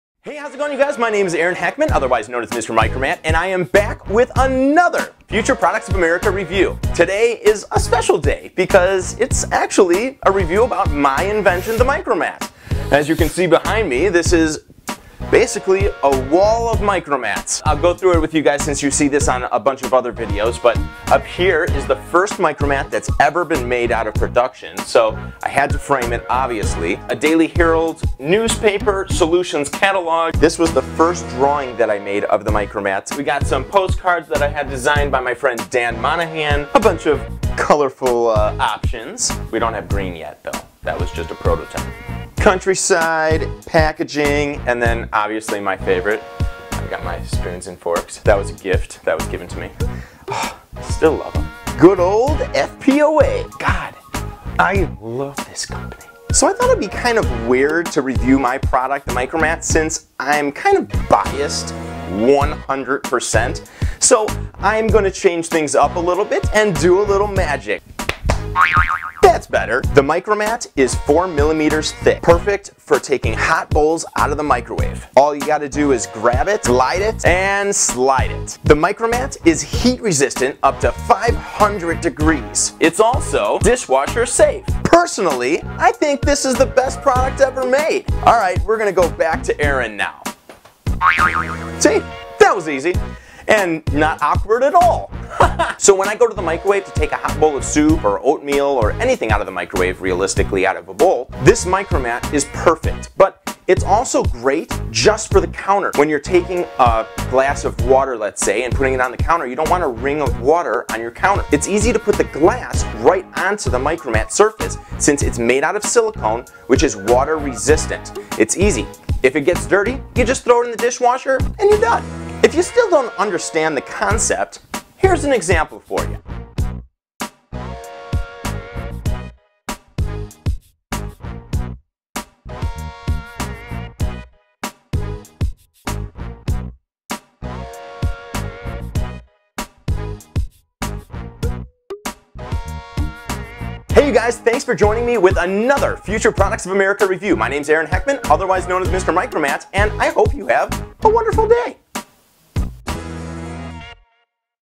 Future Products Of America (The MycroMat Review)
Future products of America presents Aaron Hechtman reviewing the MycroMat.